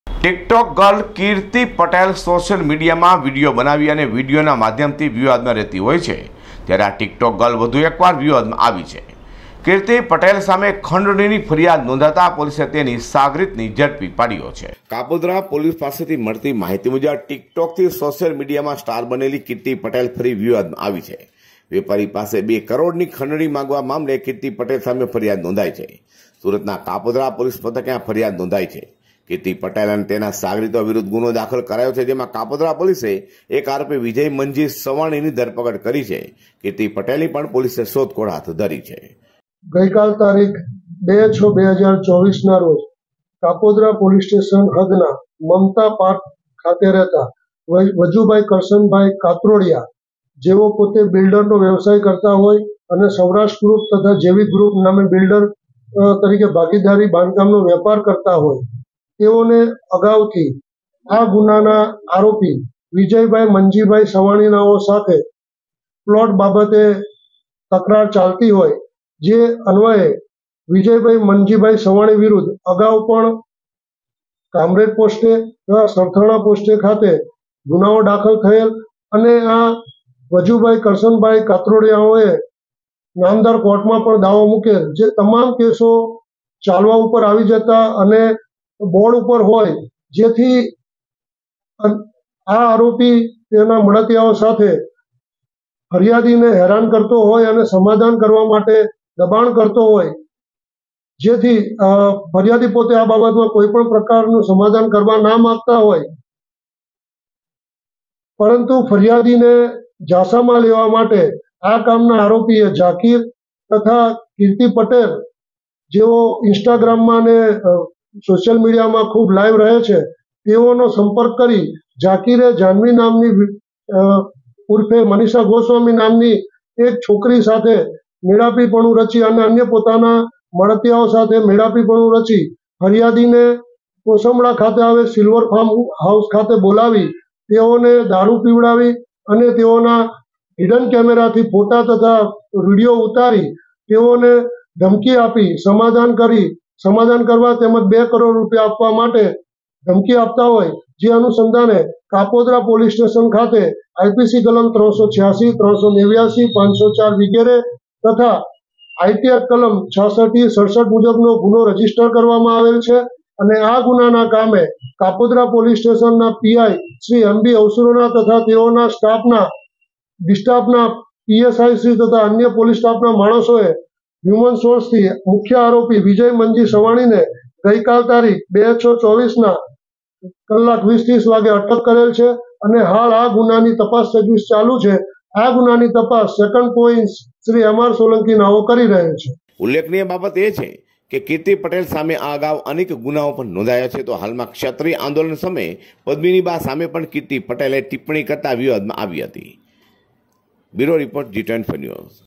કિર્તિ પટેલ સામે બે કરોડની ખંડણીની ફરિયાદ
કિર્તિ પટેલના સાગરીતને કાપોદ્રા પોલીસે ઝડપી પાડ્યો